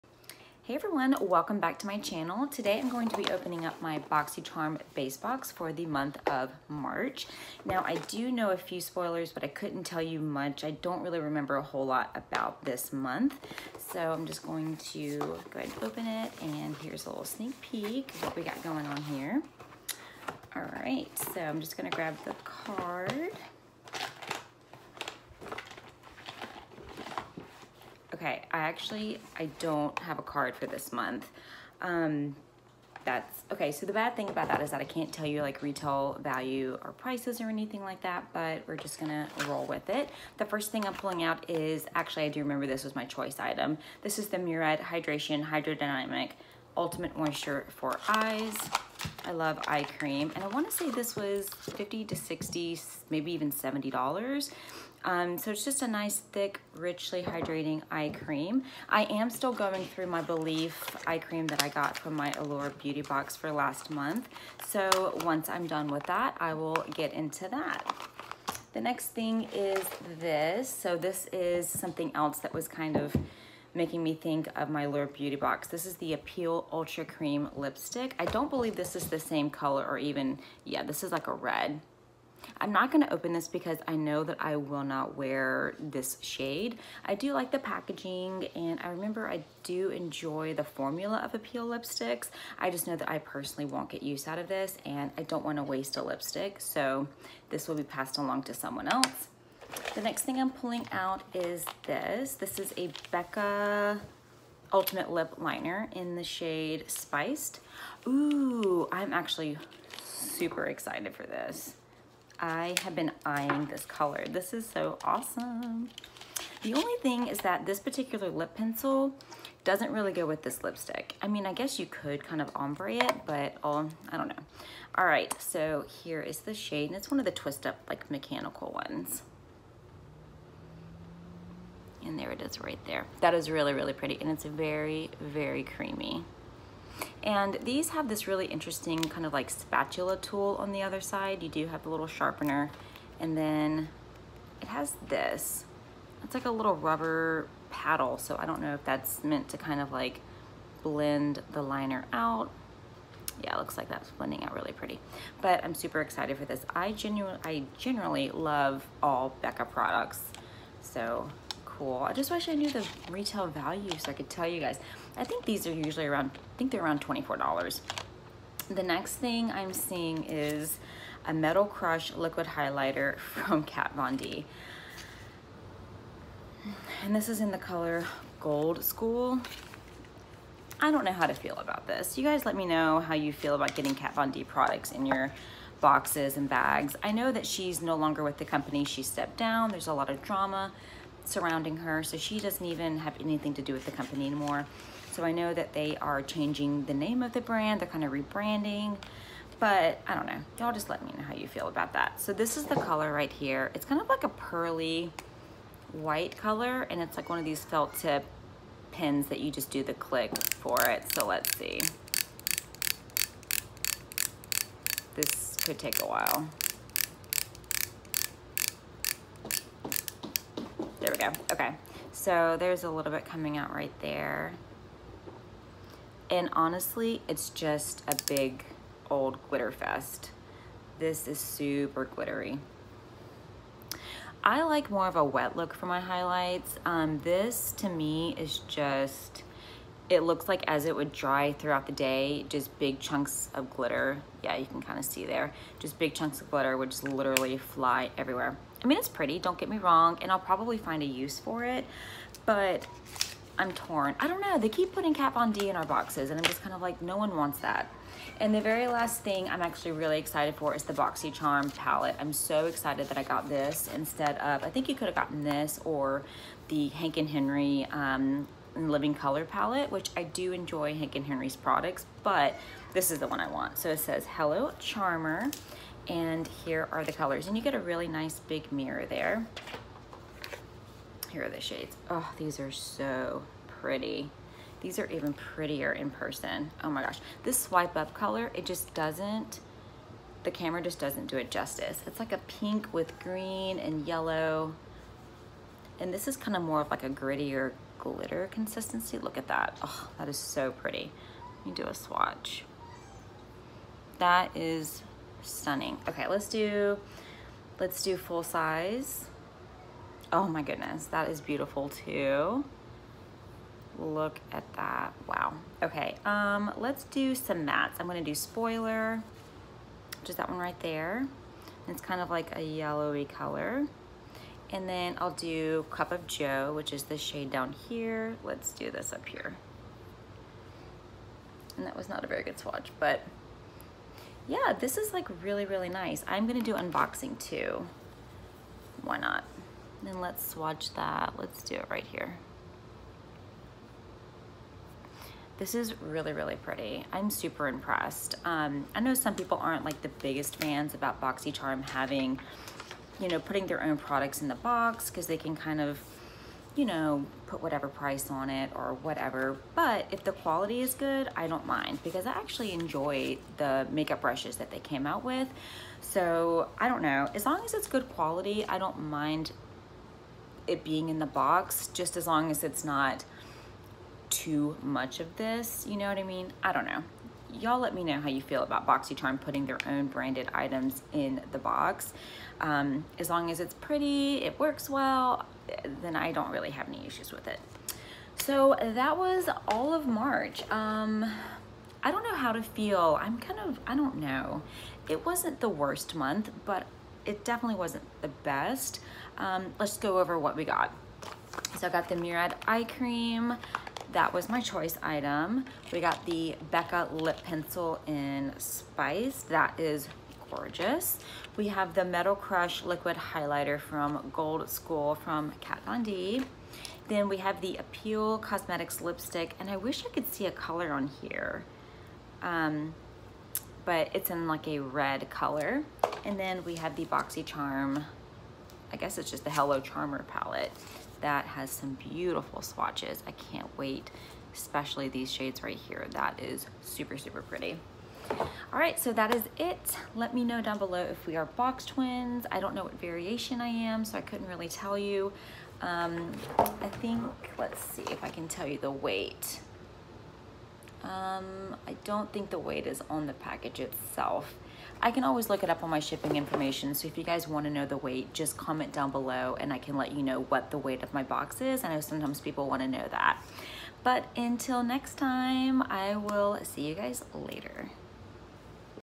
BoxyCharm Base Box for March 2020
I don’t know how I feel about this months box! Some good and some meh. Lemme know down below what you got! Sorry for no prices as I didn’t receive a card this month.